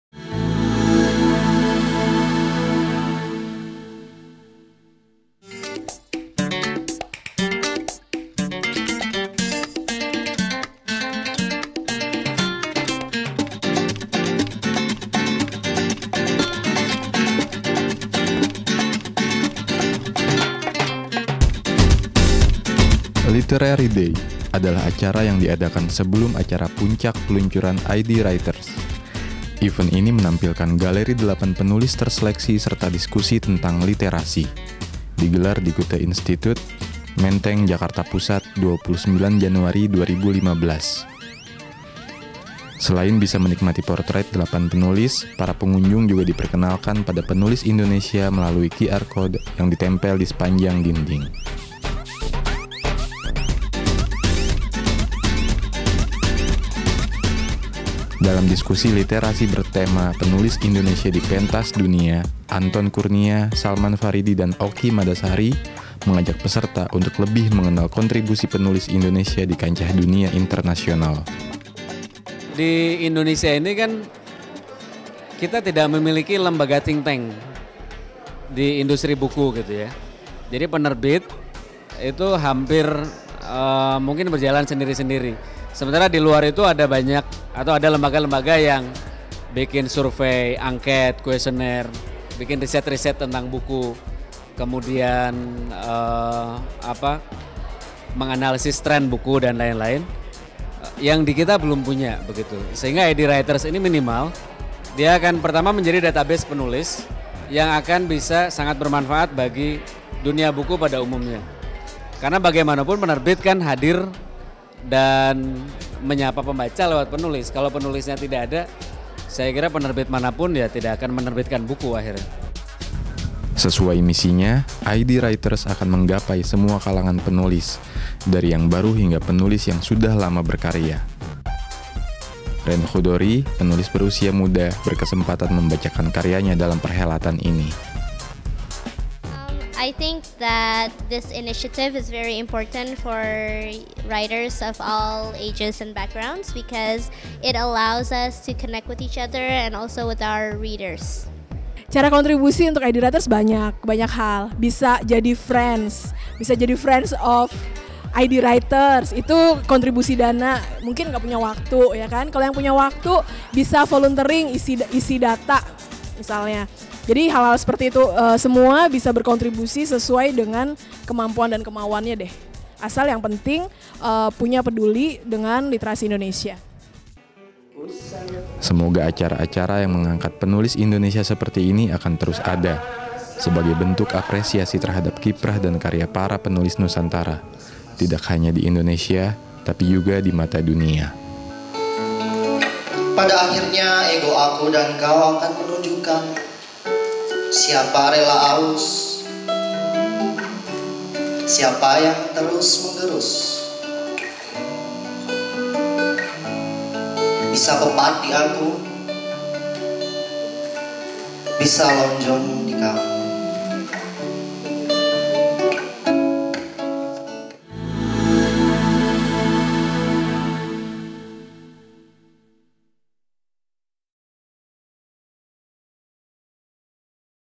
Penulis Indonesia Awali Langkah di Pentas Dunia
Sebelum acara puncak peluncuran situs IDWriters.com yang mengemban misinya Indonesian Writers for The World, digelar dua acara seru lainnya yaitu Pameran Portraits Delapan Penulis Indonesia oleh Tanam Ide Kreasi dan acara Kamis Literasi bertema Penulis Indonesia di Pentas Dunia oleh Nyonyabuku.

Pada pameran portraits delapan penulis Indonesia ini digelar barisan barcode tertempel di dinding di mana jika kita men-scan barcode tersebut menggunakan aplikasi QR Code, maka akan tertuju ke satu nama penulis yang ada di situs IDWriters.com.

Diskusi Kamis Literasi oleh Nyonyabuku menghadirkan penulis Okky Madasari dan Anton Kurnia, juga CEO dari Bentang Pustaka, Bapak Salman Faridi.